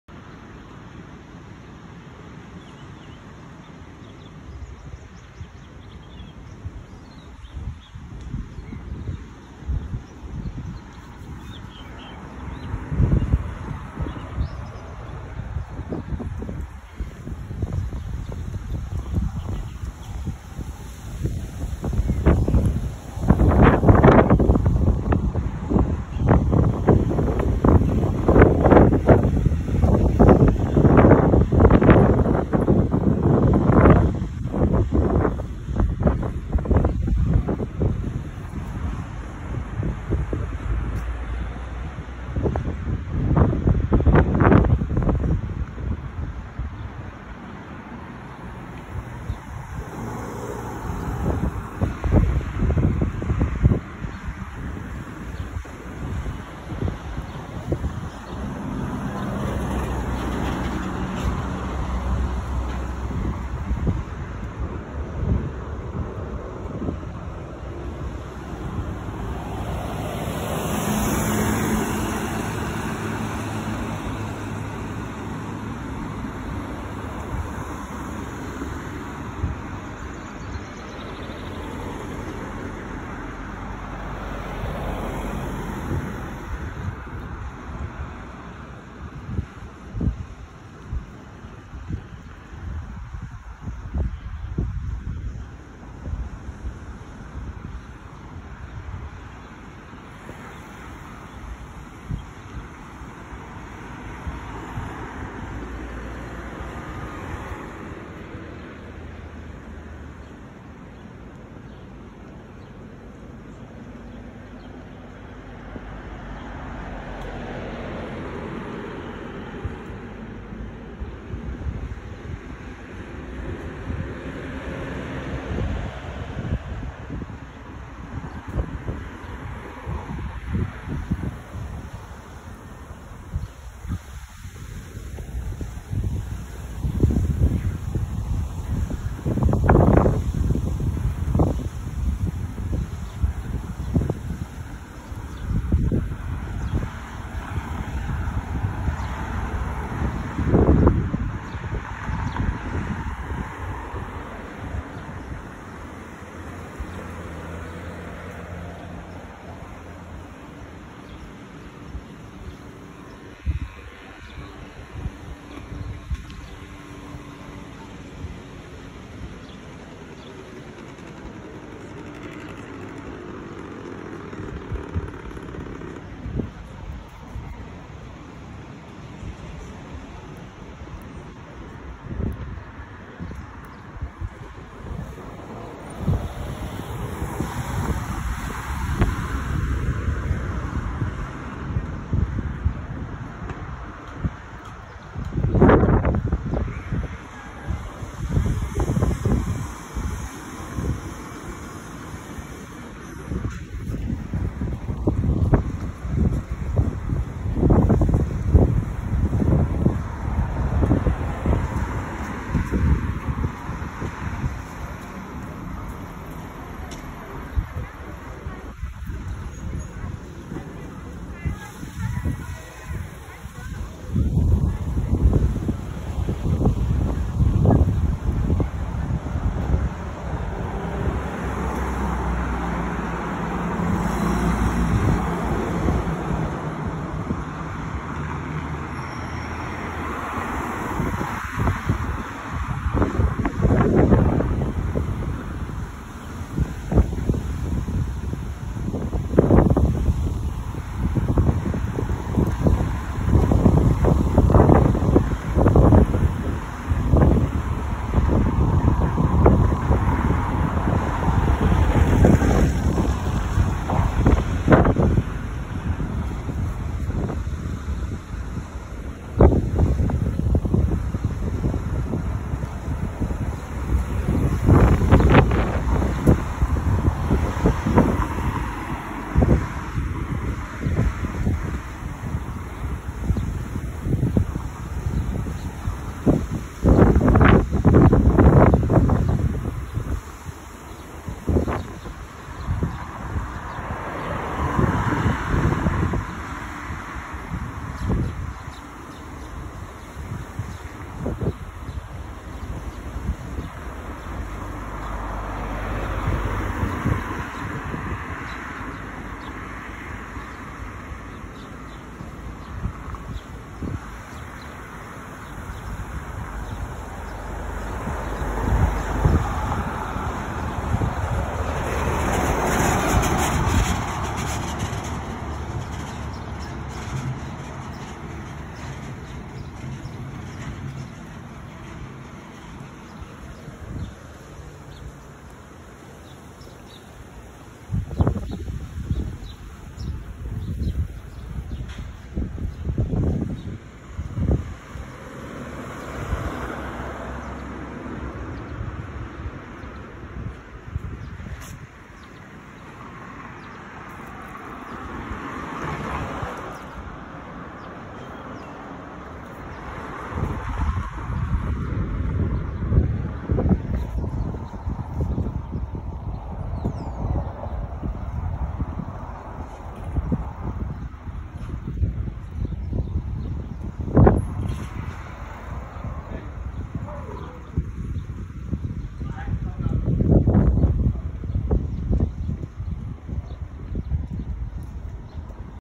Orewa main road Shops September 2021
Walking down Hibiscus Coast Highway just after breakfast in Level 3 Lockdown.